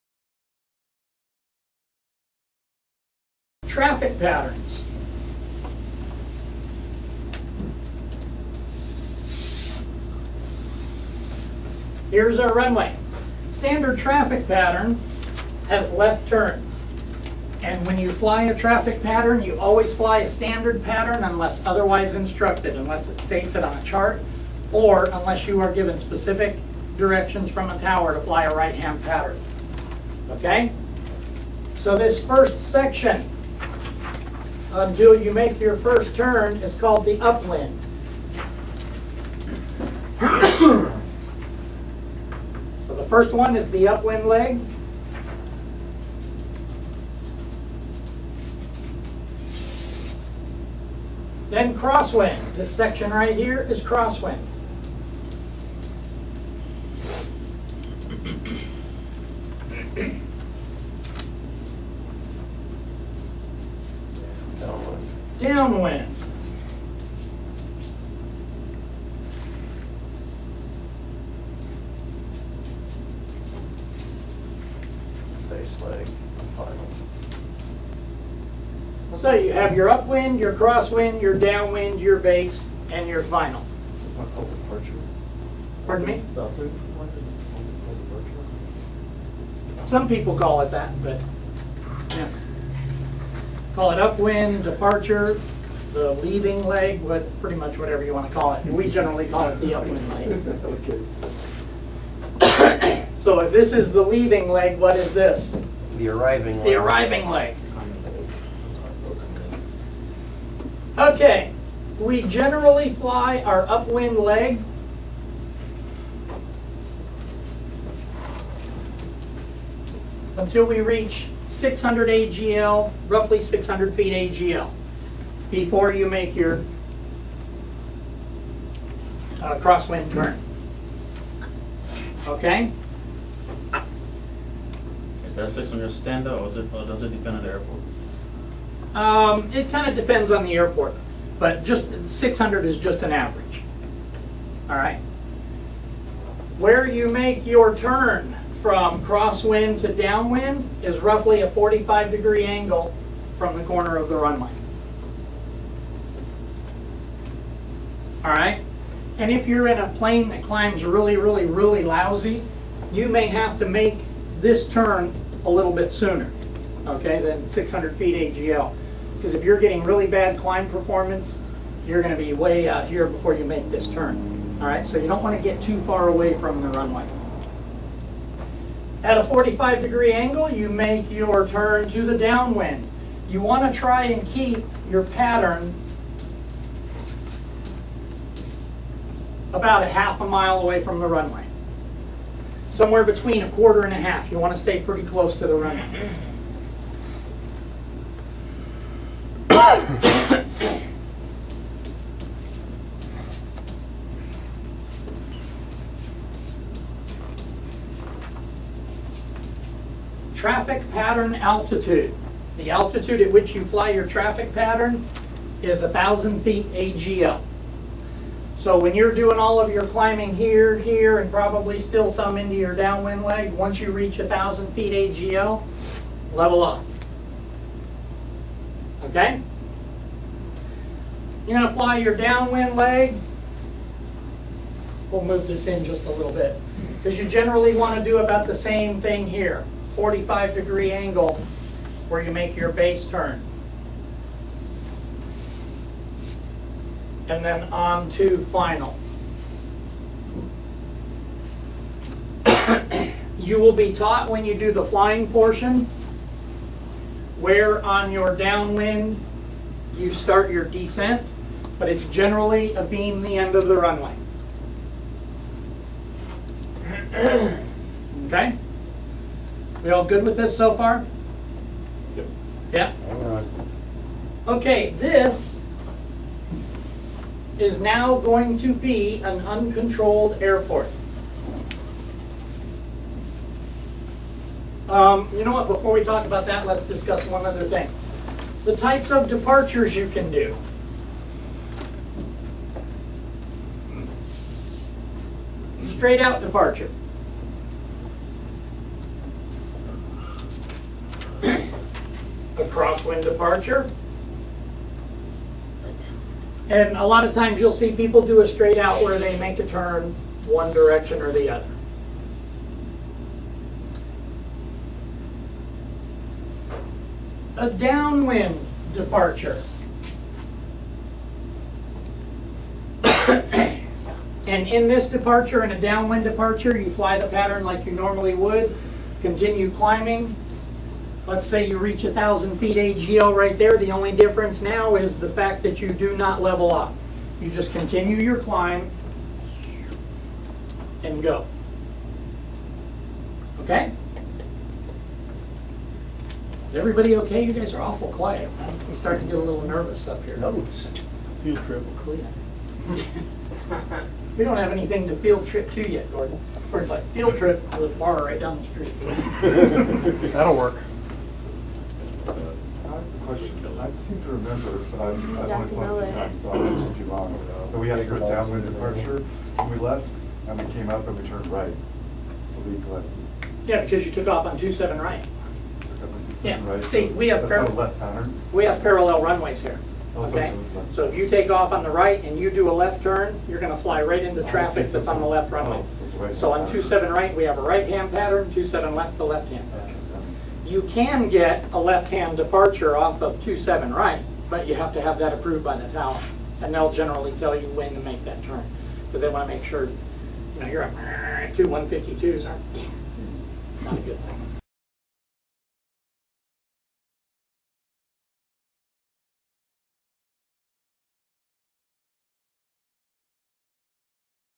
Traffic Patterns at Airports with a Control Tower
Private Pilot Training, Ground Schoo at Gillepsie Field at El Cajon, Calfornia.  See us at www dot gsfc dot com.